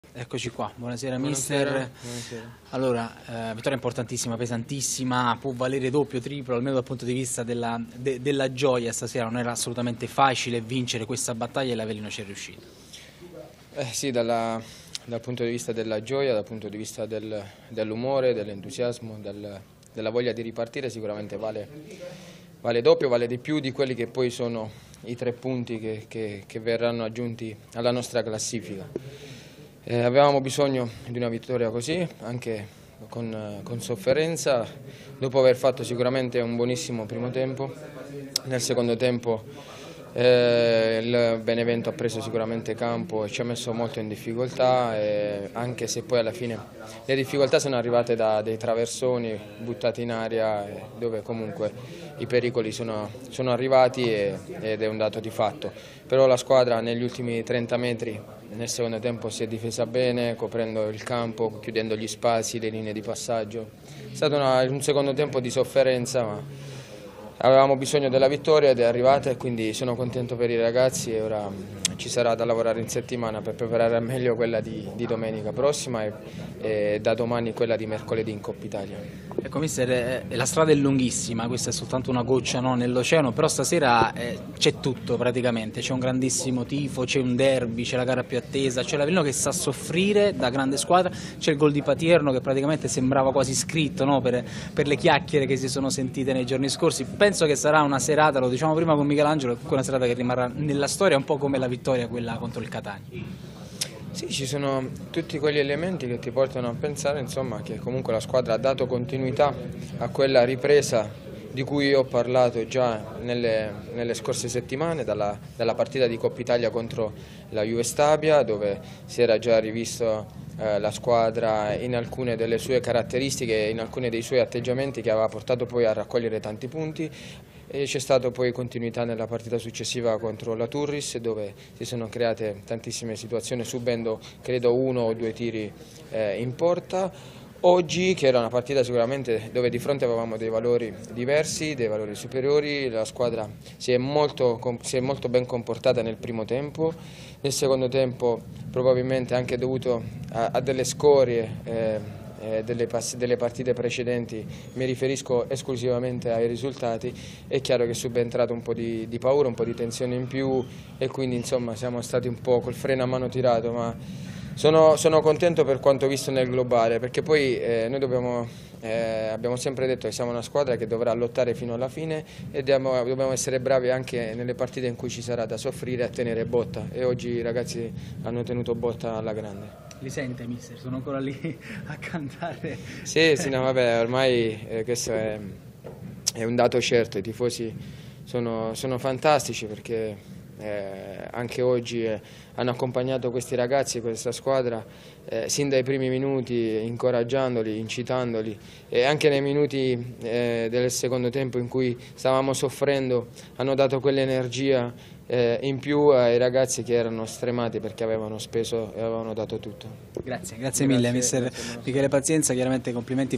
Benevento battuto, Mister Pazienza nel post gara in diretta su Prima Tivvù
L’allenatore dei lupi ha parlato in diretta su Prima Tivvù Canale 17, nel post gara di Benevento-Avellino